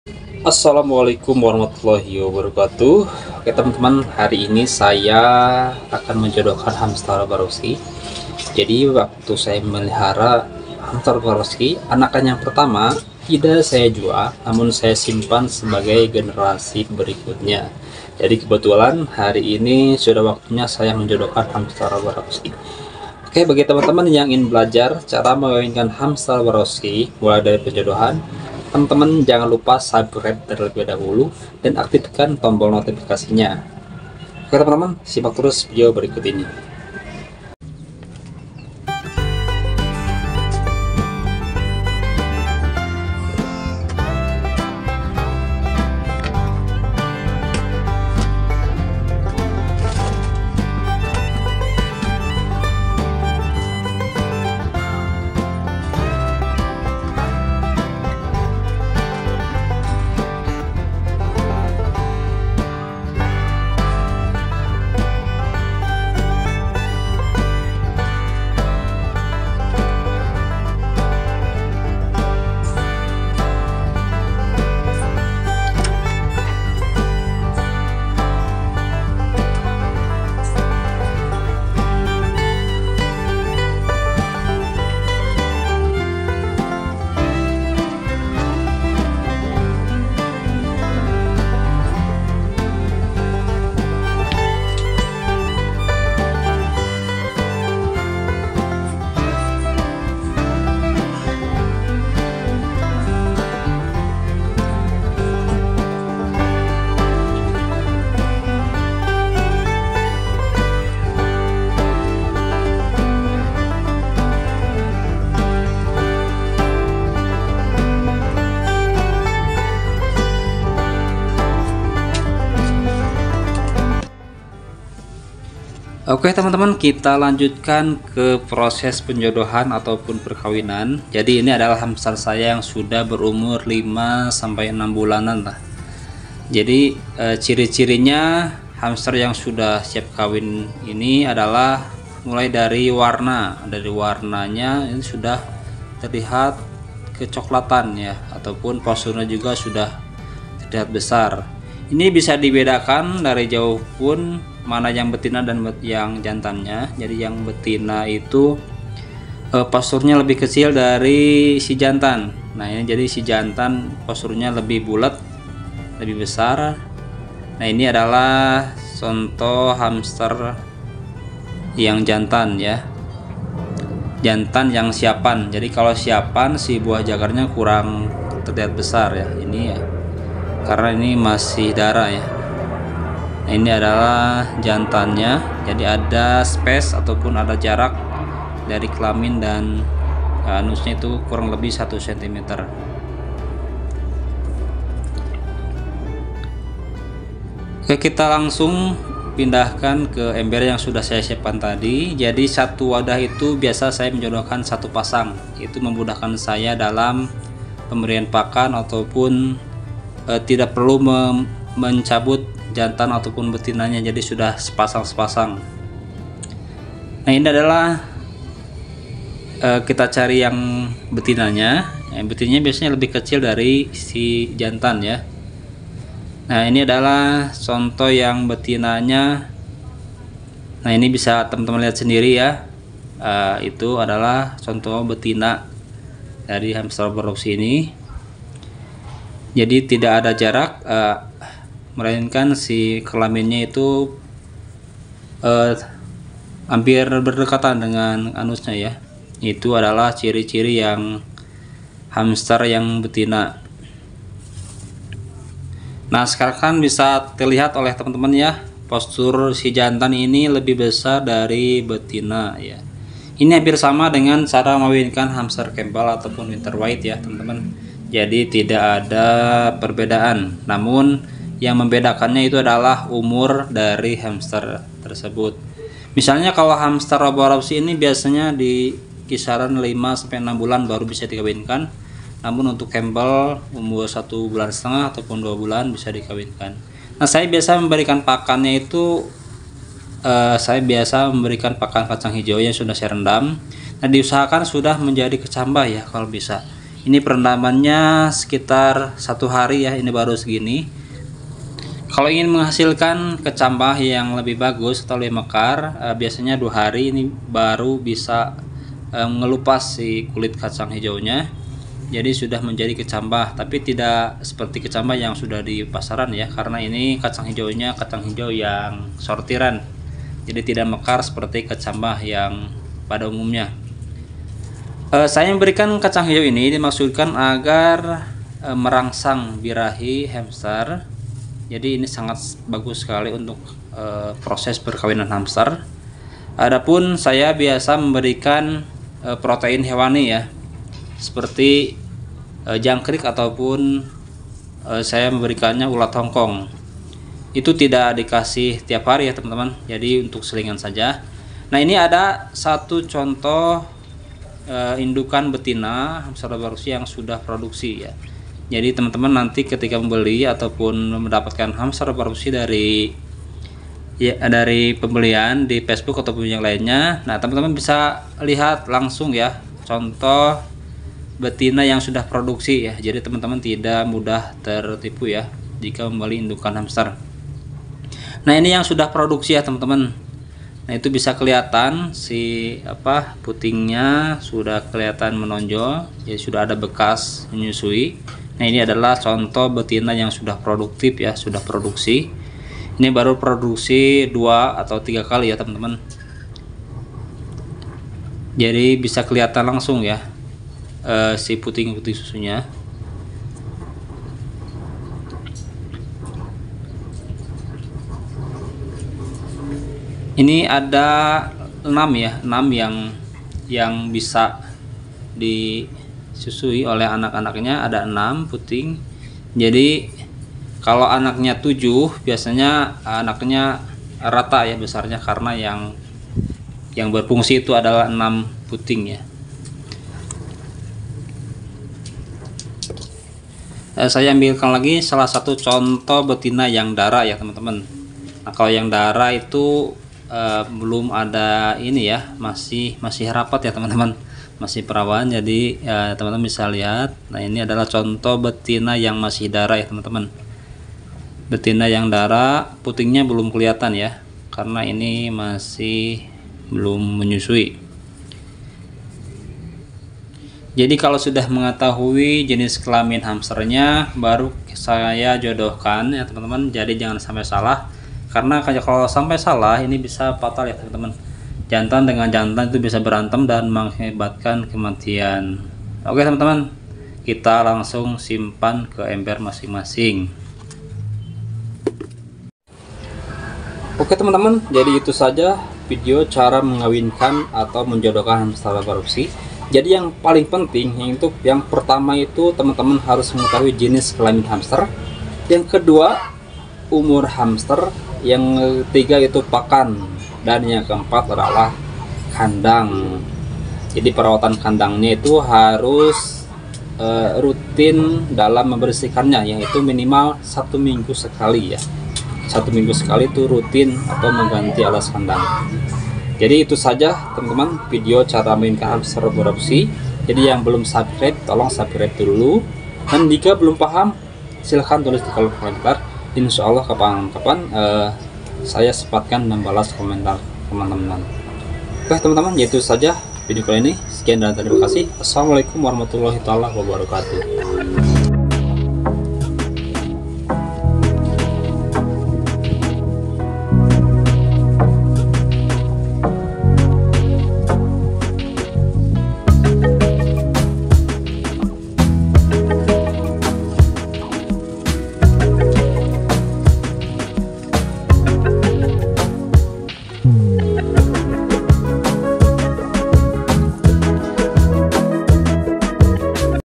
CARA MENGAWINKAN HAMSTER ROBOROVSKI BAGI PEMULA
Hallo teman teman pecinta hamster dimana pun berada semoga sehat selalu dan di mudahkan rezekinya, Amiiin

Cara menjodohkan/mengawinkan hamster roborovski hampir sama dengan cara mengawinkan hamster Campbell. perawatannya mudah dan sederhana, namun yang membedakannya yaitu pola pakan dan umur hamster, untuk masa produktif nya juga sama dengan hamster lainnya yaitu 8 kali melahirkan atau lebih. tergantung cara kita memeliharanya.